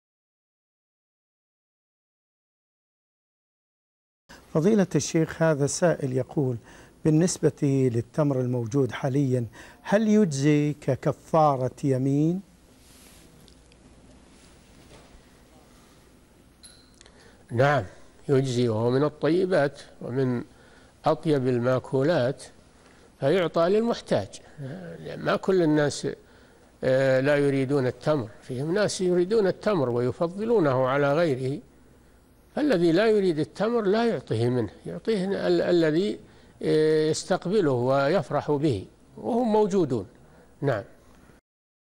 هل يجزئ التمر في كفارة اليمين ؟ - الشيخ صالح الفوزان
نص السؤال: فضيلة الشيخ! هذا سائل يقول: بالنسبة للتمر الموجود حالياً هل يجزئ ككفارة يمين؟
السيرة الذاتية :
للاشتراك في قناة الشيخ صالح الفوزان
لمشاهدة حلقات فتاوى على الهواء كاملة

رئيس الفريق
لمشروع كبار العلماء